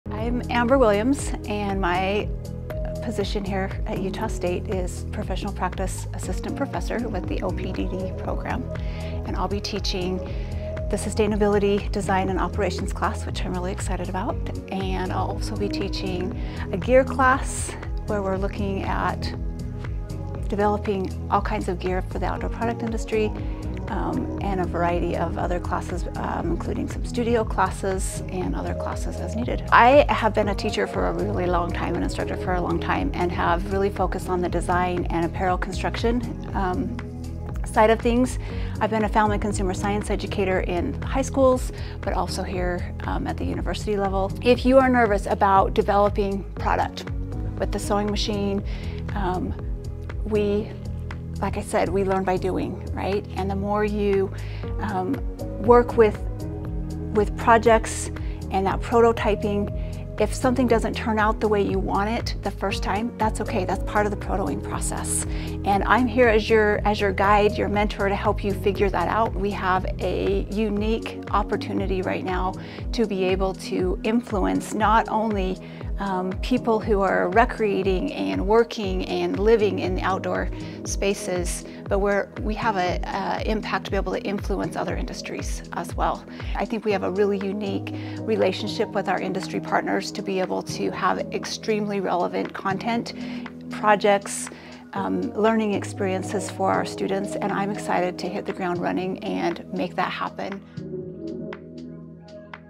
Dr. Amber Williams Joins OPDD Faculty
Utah State University is excited to announce the addition of Dr. Amber Williams as the newest member of the Outdoor Product Design & Development faculty. Dr. Williams brings a wealth of teaching experience to the program, as well as a strong expertise in apparel and soft goods design and construction.

Dr. Williams' dissertation focused on sustainability education, and she is eager to bring that focus into the classes she will be teaching, including Sustainable Design and Operations, Design Studios, among other courses. This emphasis on sustainability aligns with the program's mission to produce graduates who are well-prepared to design and develop innovative and sustainable outdoor products that meet the needs of consumers and the environment.

Dr. Williams has a deep understanding of the principles of sustainable design and how they can be applied to outdoor product development. She will be working closely with students to teach them how to design products that are not only functional and aesthetically pleasing, but also circular and socially responsible.

The Outdoor Product Design & Development program at Utah State University is known for its strong emphasis on hands-on learning and real-world experience. With Dr. Williams' addition to the faculty, students will have even more opportunities to gain valuable skills and knowledge that will prepare them for successful careers in the outdoor industry.

The university and the program are thrilled to have Dr. Williams on board and are looking forward to the contributions she will make to the program and the students. With her expertise and passion for sustainability and design, Dr. Williams is sure to be an asset to the program and the university.